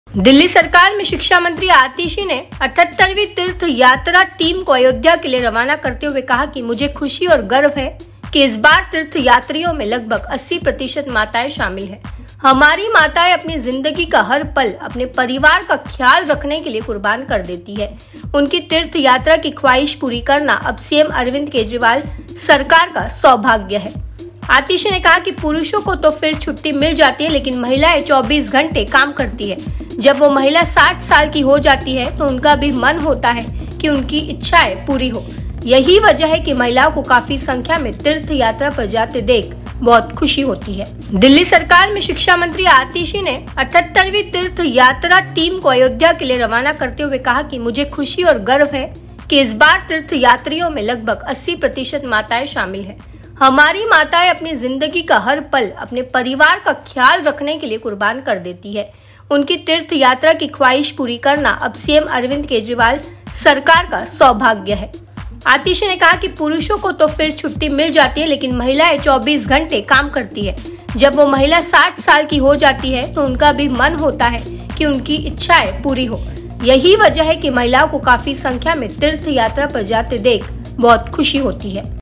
तीर्थ यात्रियों में लगभग 80% माताएं शामिल, आतिशी ने जताई ख़ुशी
दिल्ली सरकार में शिक्षा मंत्री आतिशी (Atishi) ने 78वीं तीर्थ यात्री (Tirth Yatra Yojana) टीम को अयोध्या के लिए रवाना करते हुए कहा कि मुझे खुशी और गर्व है कि इस बार तीर्थ यात्रियों में लगभग 80% माताएं शामिल हैं. हमारी माताएं अपनी जिंदगी का हर पल अपने परिवार का ख्याल रखने के लिए कुर्बान कर देती हैं. उनकी तीर्थ यात्रा की ख्वाहिश पूरी करना अब सीएम अरविंद केजरीवाल (Arvind Kejriwal) सरकार का सौभाग्य है. आतिशी ने कहा कि पुरुषों को तो फिर छुट्टी मिल जाती है, लेकिन महिलाएं 24 घंटे काम करती हैं. जब वो महिला 60 साल की हो जाती है तो उनका भी मन होता है कि उनकी इच्छाएं पूरी हो. यही वजह है कि महिलाओं को काफी संख्या में तीर्थ यात्रा पर जाते देख बहुत खुशी होती है.